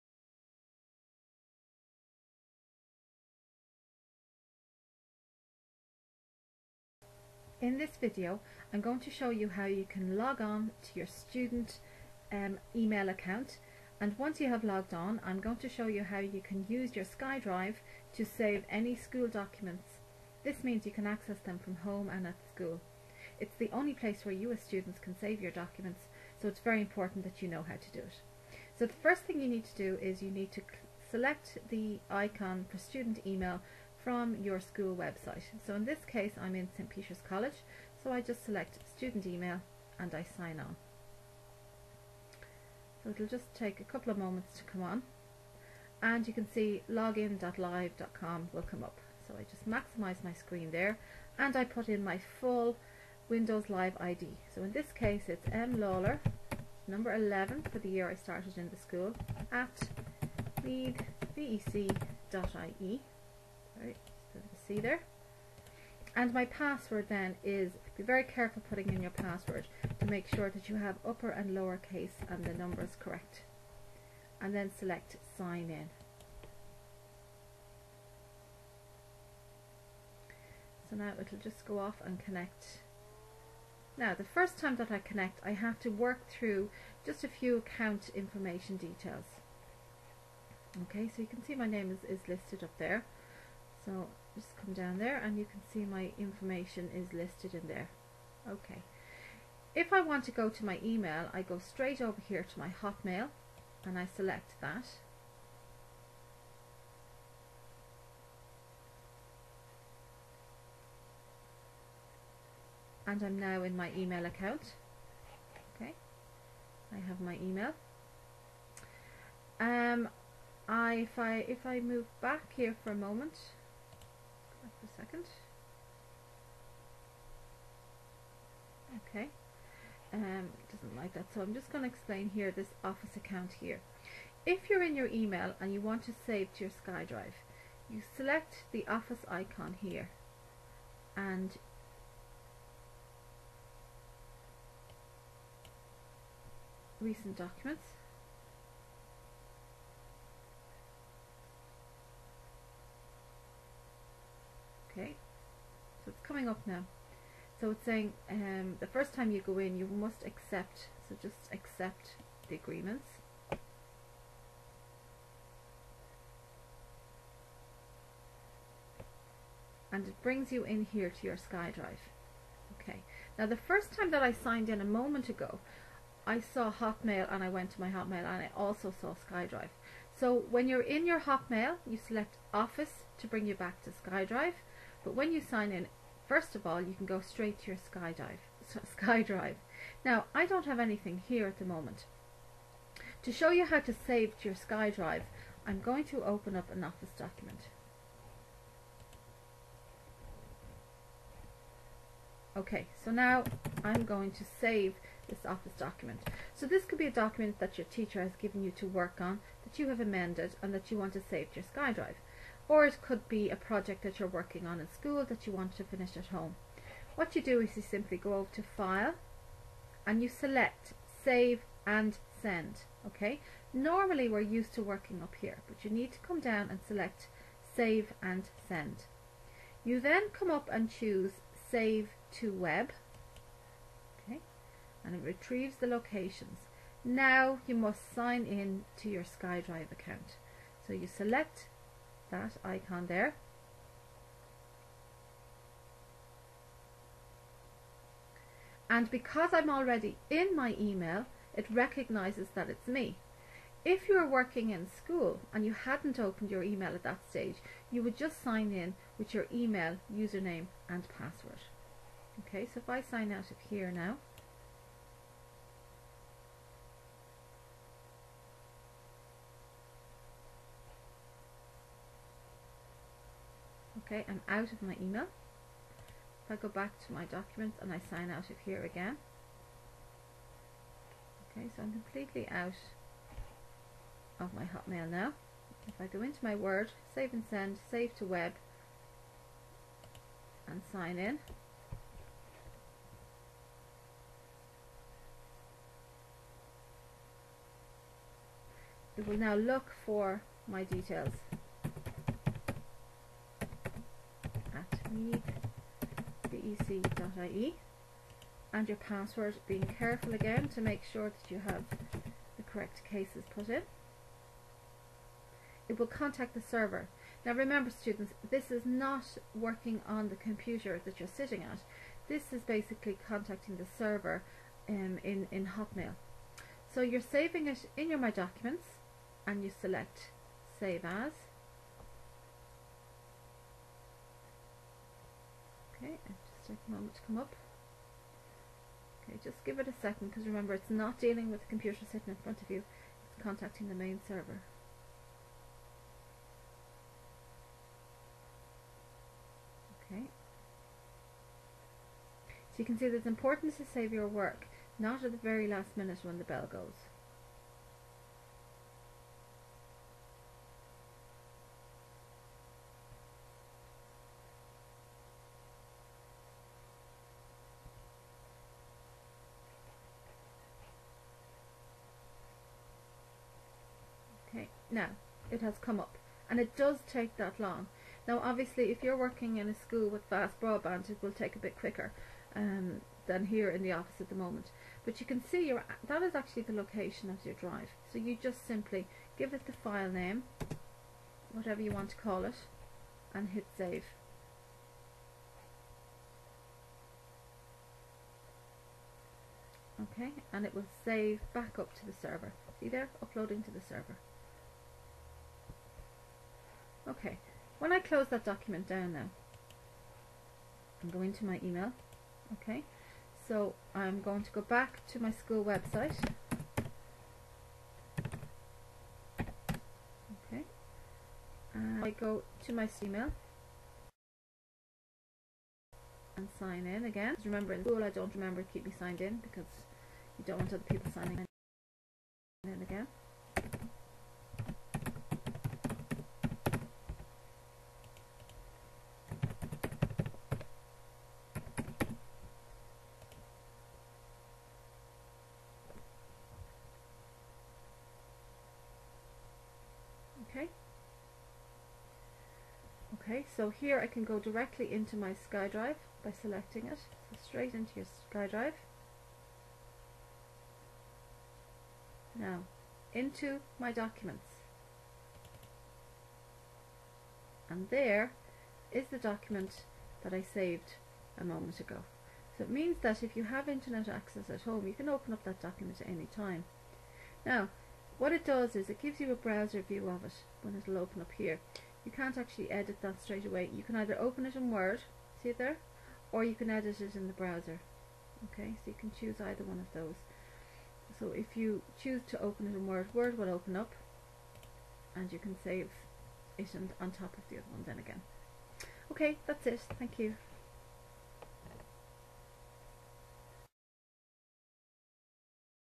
Sign in to your student email and use the SkyDrive to save your work
student email